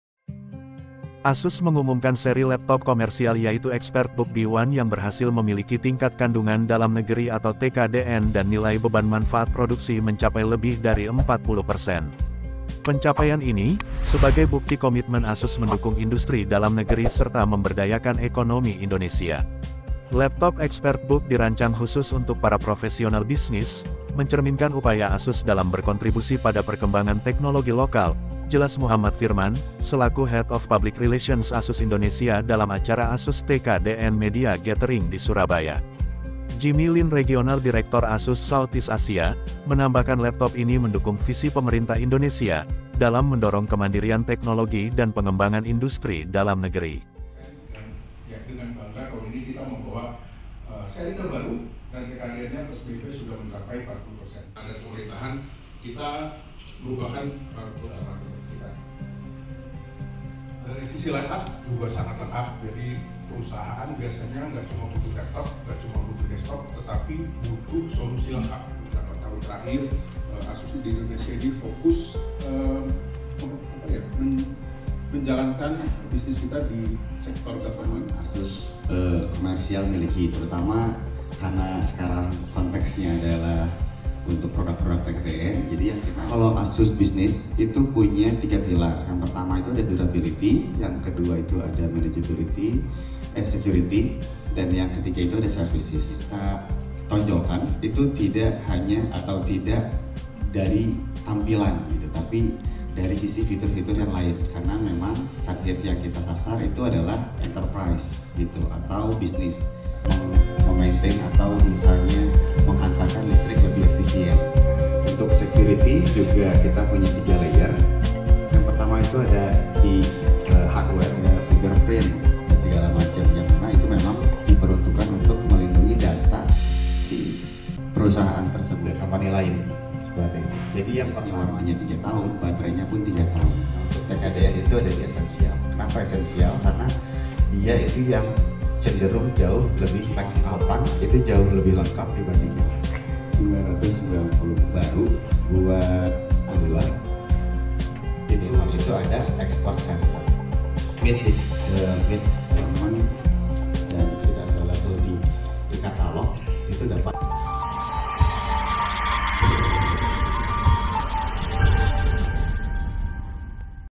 ExpertBook B1 Laptop Pertama ASUS dengan TKDN di Atas 40%
Dalam upaya mendukung program pemerintah terkait Tingkat Kandungan Dalam Negeri (TKDN) produk elektronik, termasuk laptop, ASUS telah memproduksi laptop segmen B2B, government dan education di Batam sejak akhir tahun 2022 lalu.

Sebagai bukti komitmen ASUS untuk bersama-sama pemerintah dalam membangun ekosistem industri teknologi di dalam negeri, di tahun 2024 ini ASUS berhasil mencatatkan pencapaian bersejarah.

Lewat produk laptop terbarunya, kini ASUS berhasil mencatatkan angka di atas 40% TKDN dan BMP.

Pada kesempatan kali ini, ASUS Indonesia menggelar acara TKDN Media Gathering: "ASUS Semakin Indonesia, Raih 40% TKDN untuk Laptop", Kamis, 19 September 2024, di Kalingga Meeting Room
Sheraton Hotel Tunjungan Surabaya.